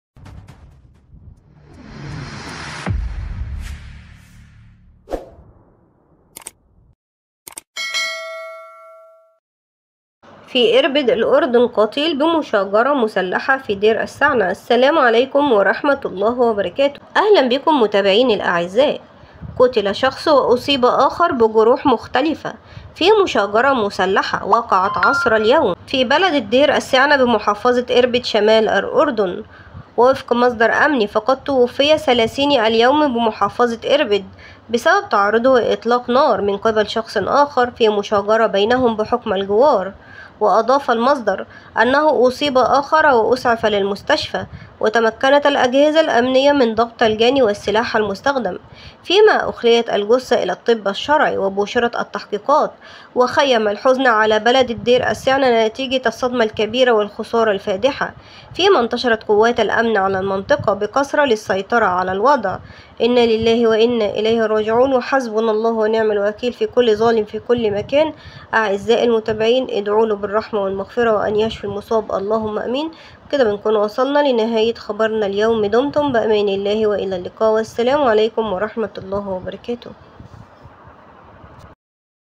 تفاصيل جريمه قتل عبد الله ومحمد حسين الهياجنه في دير سعنه بالاردن في مشاجره اربد الاردن
مقتل عبدالله ومحمد حسين الهياجنه في دير السعنه يهز الاردن

دير السعنه
دير السعنة
اربد
عبدالله حسين الهياجنه
محمد حسين الهياجنه
الاردنقتل شخص وأصيب آخر بجروح مختلفة في مشاجرة مسلحة وقعت عصر اليوم الاربعاء في بلدة دير السعنة بمحافظة اربد شمال الاردن .

ووفق مصدر امني، فقد توفي ثلاثيني اليوم بمحافظة إربد، إثر تعرّضه لإطلاق نار من قبل شخص آخر، في مشاجرة بينهما، بحكم الجوار.

واضاف المصدر: انه أصيب آخر وأسعف للمستشفى، وتمكّنت الأجهزة الأمنية من ضبط الجاني والسلاح المستخدم، فيما أخليت الجثة إلى الطب الشرعي وبوشرت التحقيقات.

وخيّم الحزن على بلدة دير السعنة، نتيجة الصدمة الكبيرة والخسارة الفادحة، فيما تنتشر قوات الامن في المنطقة بكثرة للسيطرة على الوضع .

وقد غرقت صفحات التواصل الاجتماعي بعبارات العزاء والدعوة الى التهدية والهدوء والدعاء من الله تعالى ان يحفظ الناس بدمائهم ويبعد عنهم كل مكروه .